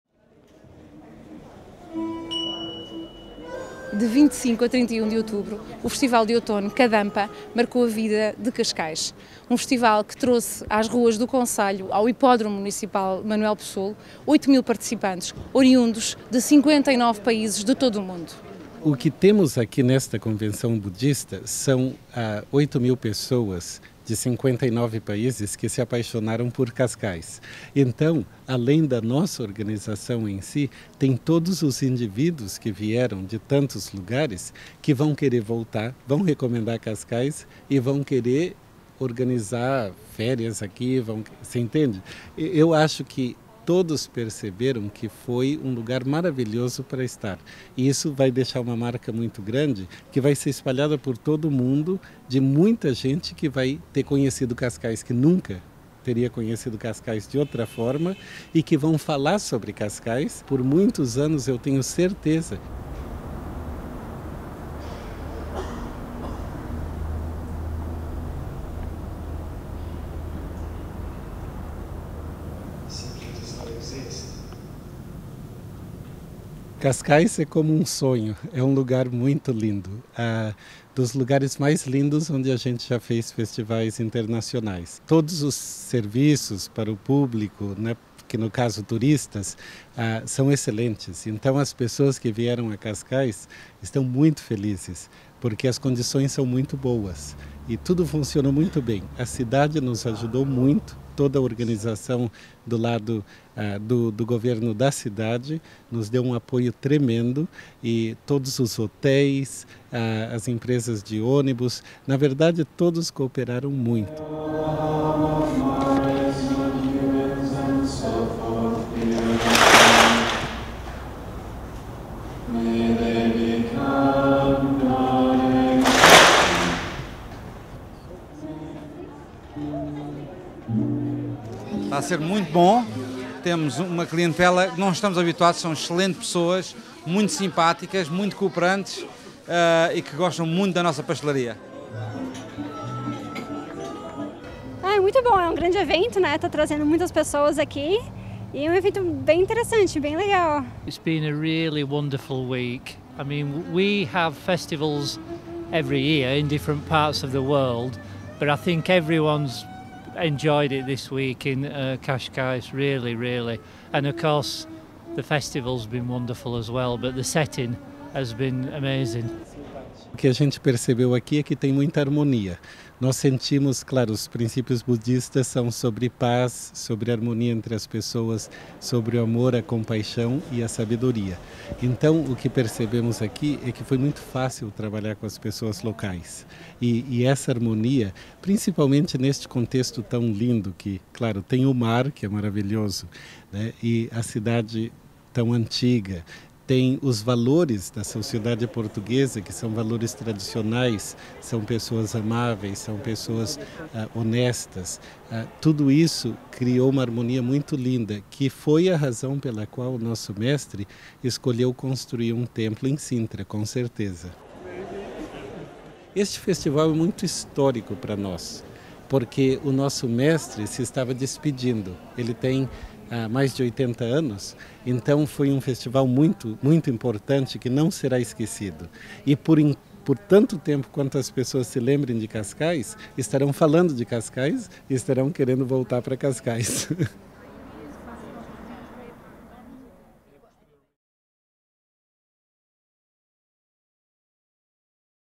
Budistas Kadampa em Cascais | Festival Internacional de Outono 2013
Câmara Municipal de Cascais
Reportagem: Fátima Henriques
Imagem e Edição: António Maria Correia